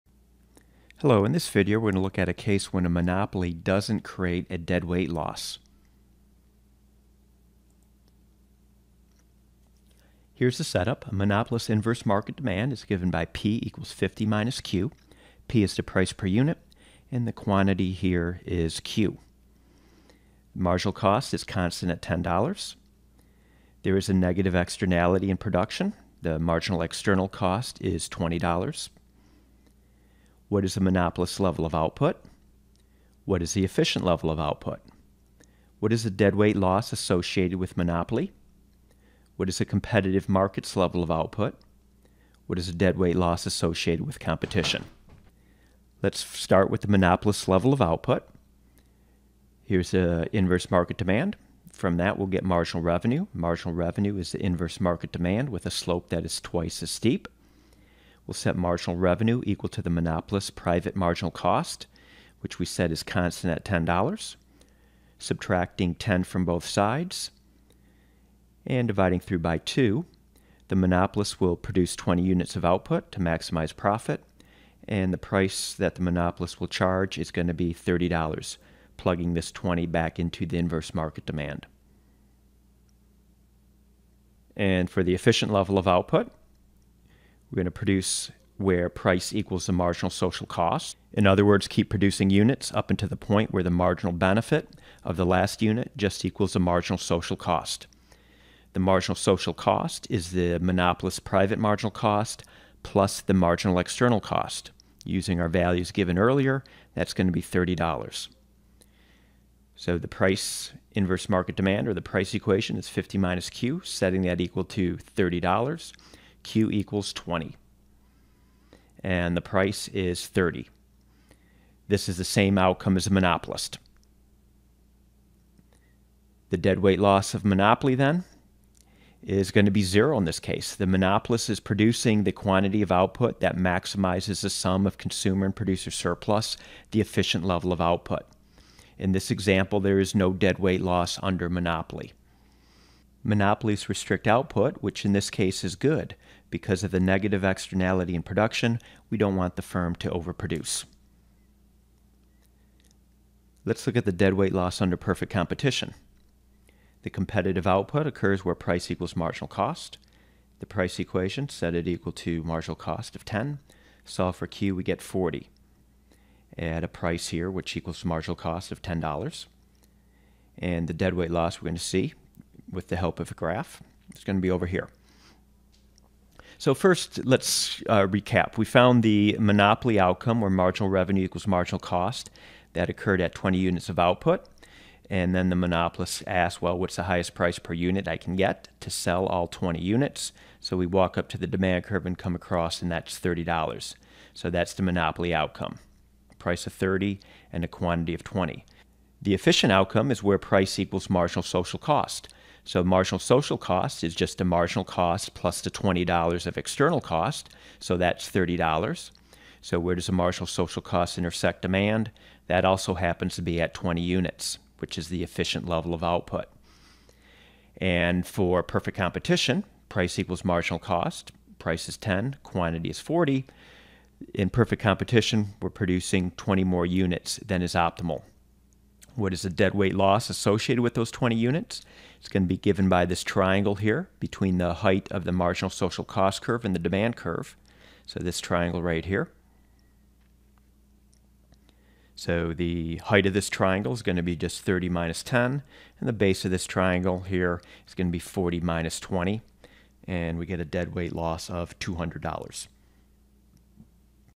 Monopoly: The Case of No Deadweight Loss
This video looks at a situation in which the monopolist produces the efficient level of output by inadvertently accounting for a negative externality in production.